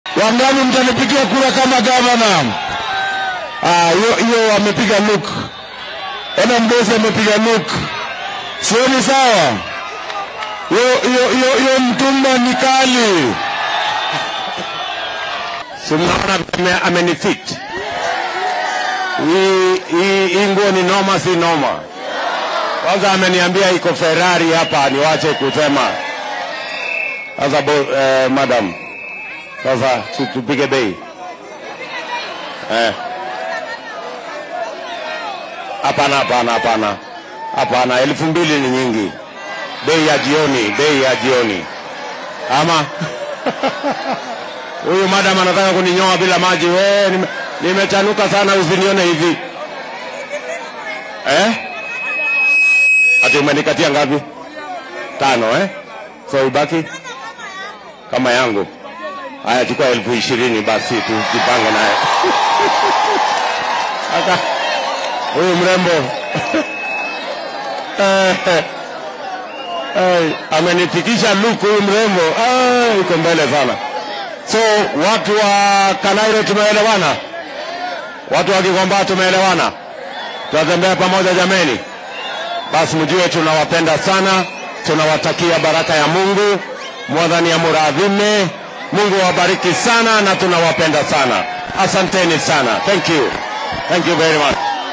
Ruto ‘bargains’ for mtumba jacket at Gikomba, buys it at Sh20,000
Ruto ‘bargains’ for mtumba jacket at Gikomba market, but ends up paying Sh20,000